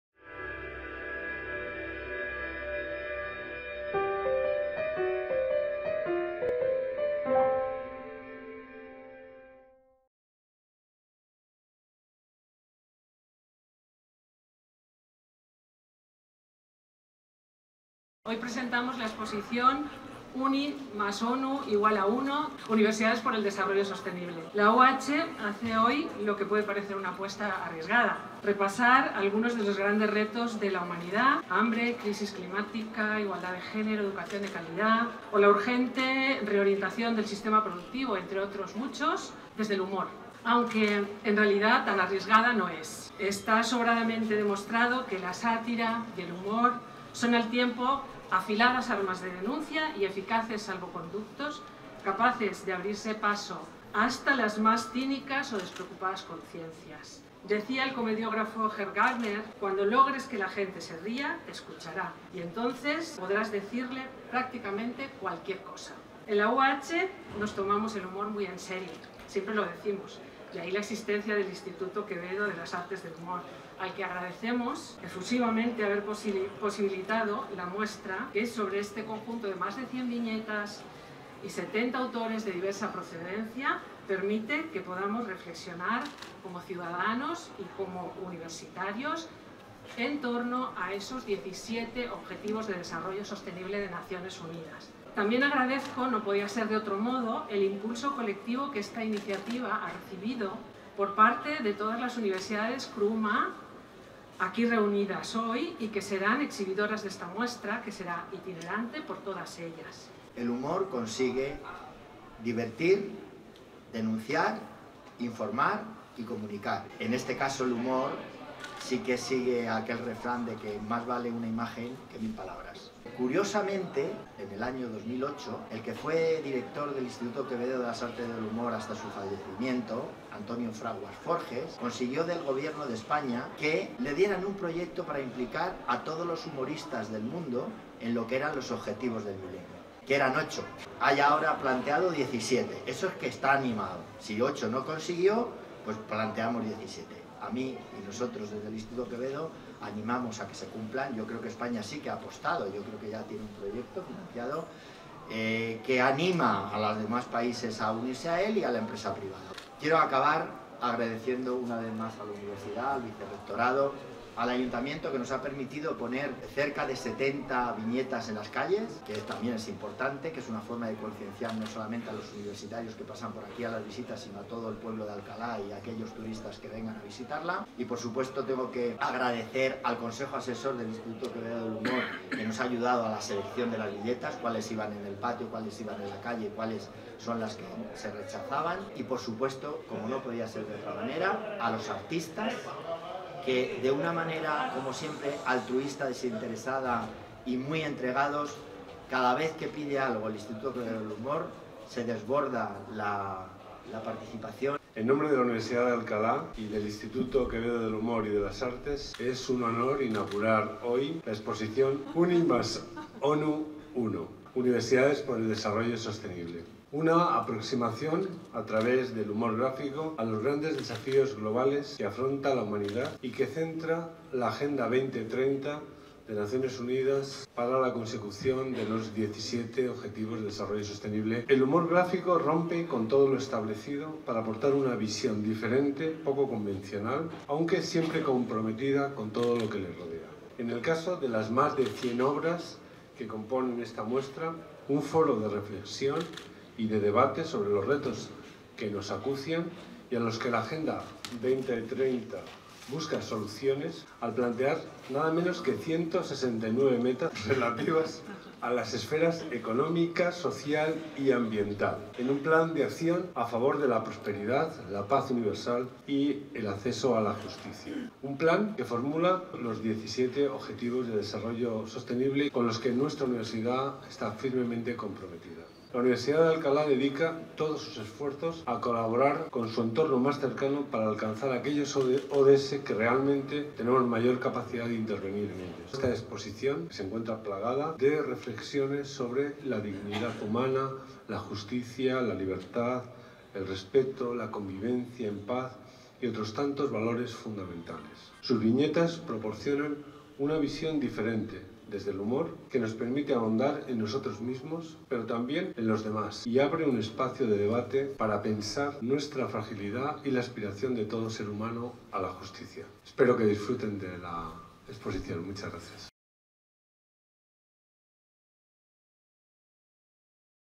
Inauguración | Exposición: 'Uni + ONU = Uno. Universidades por el Desarrollo Sostenible' (Alcalá)
Video de la inauguración de 'Uni + ONU = Uno. Universidades por el Desarrollo Sostenible', exposición que podrá visitarse hasta el 9 de febrero de 2020 en el Patio Santo Tomás de Villanueva (Rectorado UAH) y las Calles Mayor y Libreros de Alcalá de Henares. Toda la información en nuestra página web